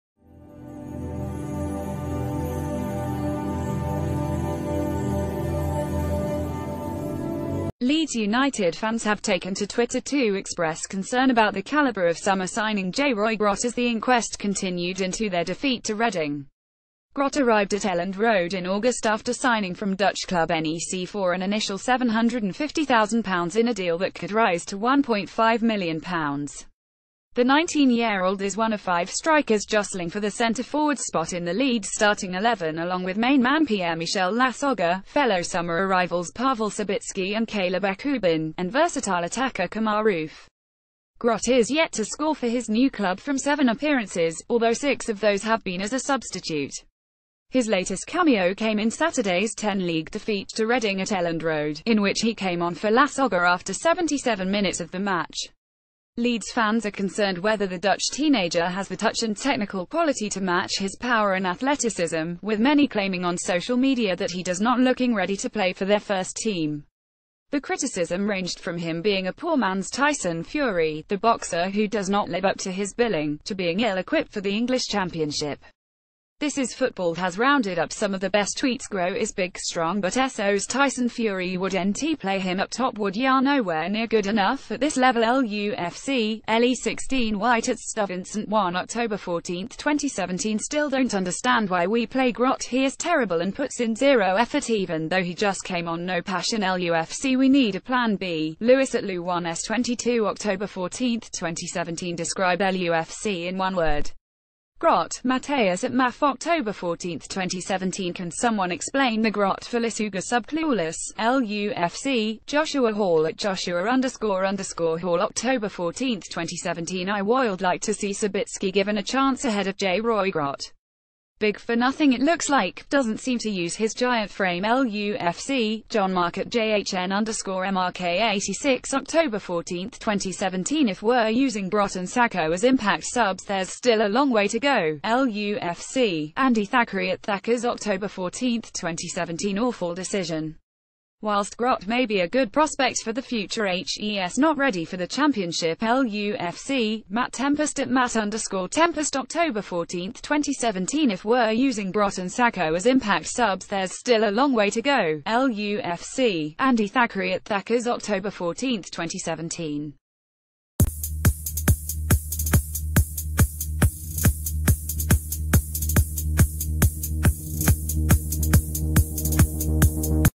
Like a poor man's tyson fury - leeds fans concerned about summer signing- [News 24h]
Like a poor man's tyson fury - leeds fans concerned about summer signing
Copyright@ by News24h
Leeds United fans have taken to Twitter to express concern about the calibre of summer signing JayRoy Grot as the inquest continued into their defeat to Reading. Grot arrived at Elland Road in August after signing from Dutch club NEC for an initial £750,000 in a deal that could rise to £1.5million. The 19yearold is one of five strikers jostling for the centreforward spot in the Leeds starting XI along with main man PierreMichel Lasogga, fellow summer arrivals Pawel Cibicki and Caleb Ekuban, and versatile attacker Kemar Roofe. Grot is yet to score for his new club from seven appearances, although six of those have been as a substitute. His latest cameo came in Saturday’s 10 league defeat to Reading at Elland Road, in which he came on for Lasogga after 77 minutes of the match. Leeds fans are concerned whether the Dutch teenager has the touch and technical quality to match his power and athleticism, with many claiming on social media that he does not looking ready to play for their first ...